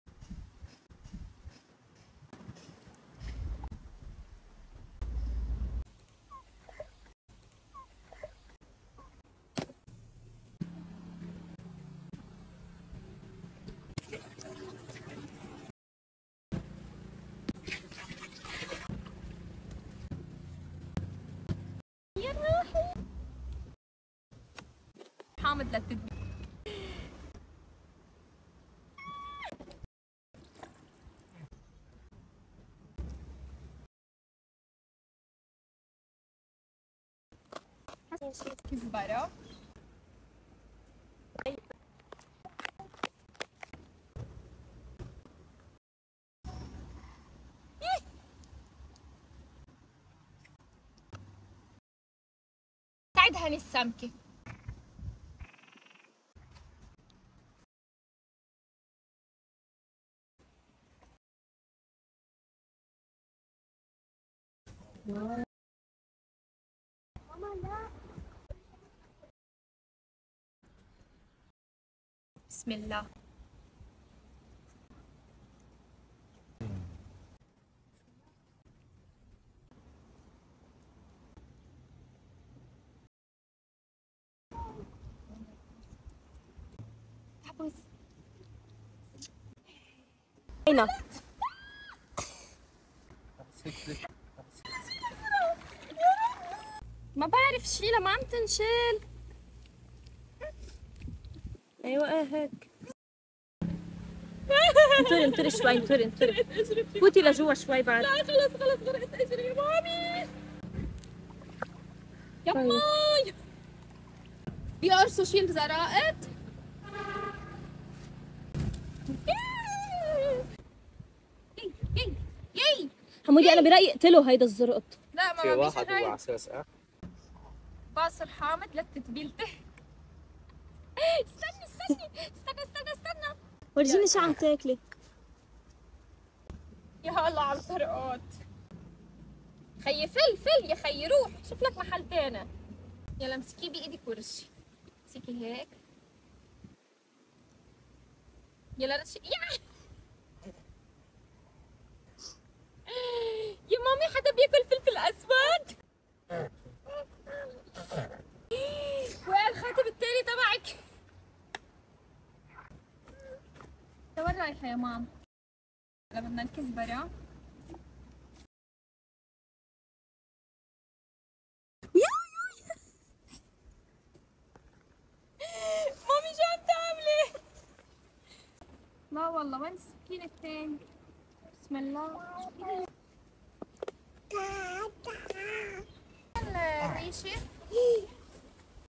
سمكة مشوية عالنهر مع ميلانا وكواليس خرافية🙀🏃🏻‍♀️
مكونات تتبيلة الحشوة:
كزبرة-ليمون(حامض)-برتقال-ملح-فلفل اسود-كركم(عقدة صفرة)- فليفلة حمرا-زيت زيتون.
مكونات تتبيلة الدهن:
ليمون(حامض)- برتقال-ملح-فلفل اسود-كركم(عقدة صفرة)-زيت زيتون.